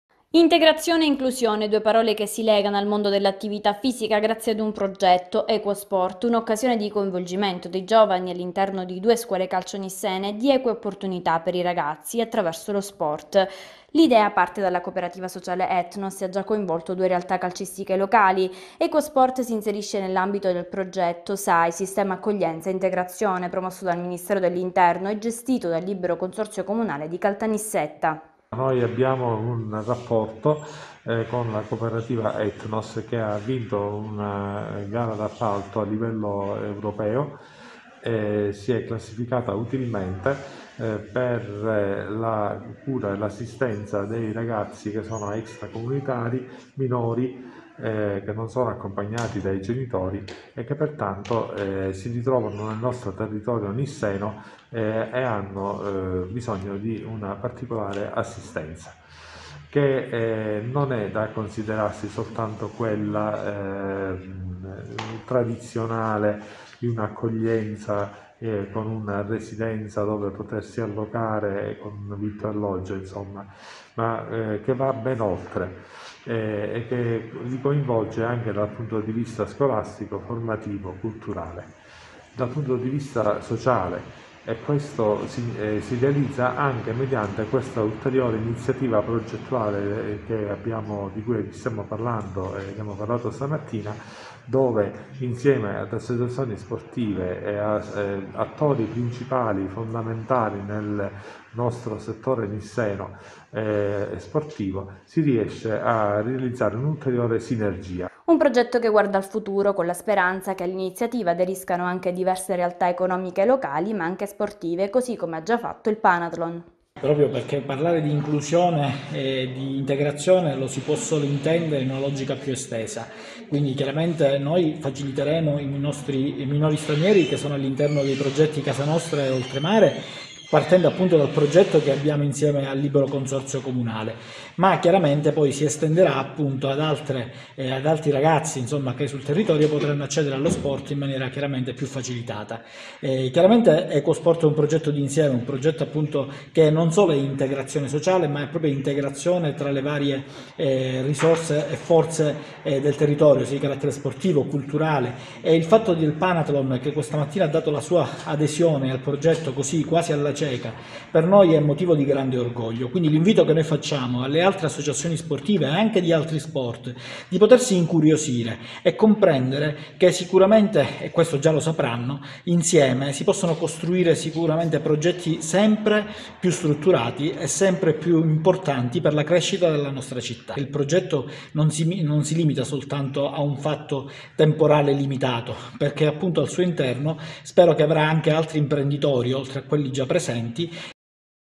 “Equo Sport” un progetto di inclusione
“Equo Sport” un progetto di inclusione che consente ai ragazzi che non se lo possono permettere di frequentare le scuole calcio